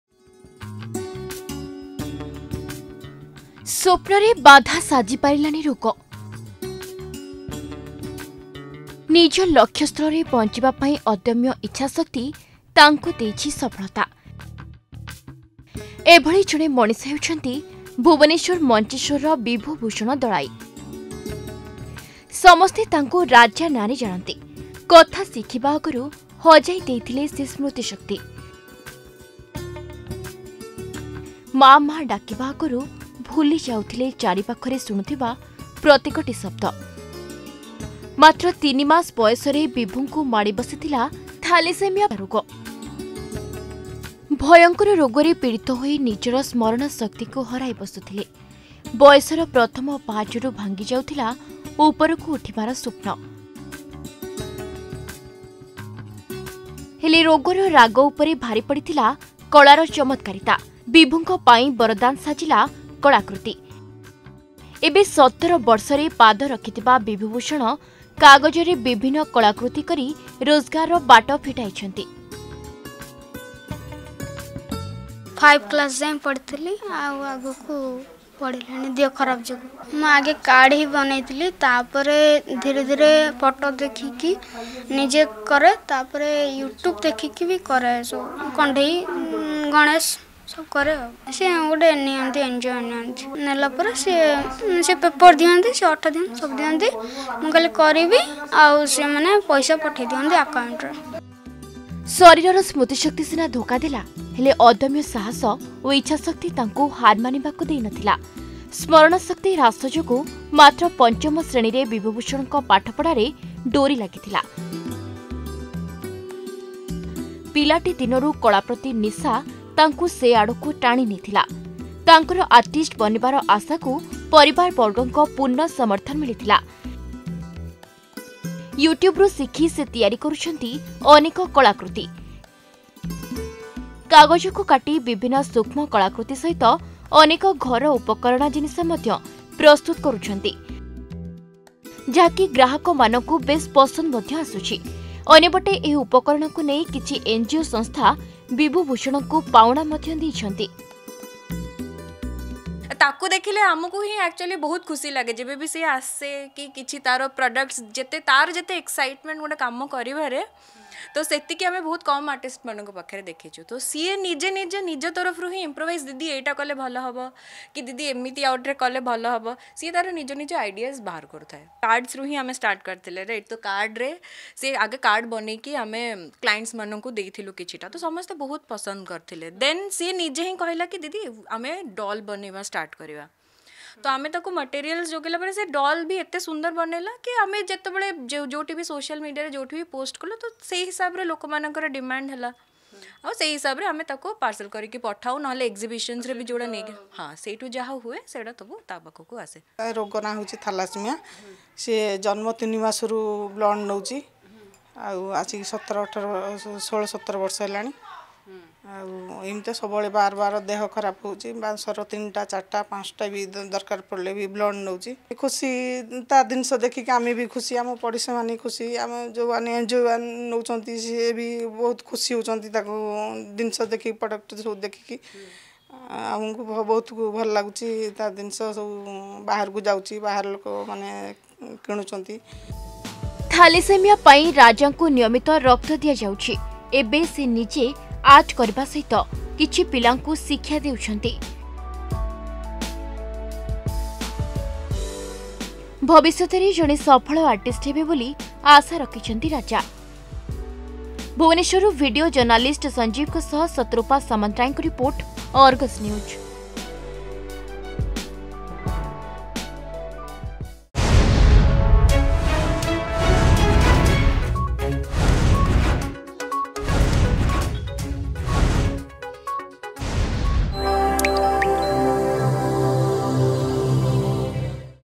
ସ୍ୱପ୍ନରେ ବାଧା ସାଜିପାରିଲାନି ରୋଗ
ସ୍ୱପ୍ନରେ ବାଧା ସାଜିପାରିଲାନି ରୋଗ । ନିଜ ଲକ୍ଷ୍ୟସ୍ଥଳରେ ପହଞ୍ଚିବା ପାଇଁ ଅଦ୍ୟମ ଇଚ୍ଛାଶକ୍ତି ତାଙ୍କୁ ଦେଇଛି ସଫଳତା । ଏଭଳି ଜଣେ ମଣିଷ ହେଉଛନ୍ତି ଭୁବନେଶ୍ୱର ମଞ୍ଚେଶ୍ୱରର ବିଭୂଭୂଷଣ ଦଳାଇ... ସମସ୍ତେ ତାଙ୍କୁ ରାଜା ନାଁରେ ଜାଣନ୍ତି । କଥା ଶିଖିବା ଆଗରୁ ହଜାଇ ଦେଇଥିଲେ ସେ ସ୍ମୃତି  ଶକ୍ତି ...
ଅଧିକ ଖବର ପାଇଁ ଡ଼ାଉନଲୋଡ କରନ୍ତୁ “Argus News” ଆପ୍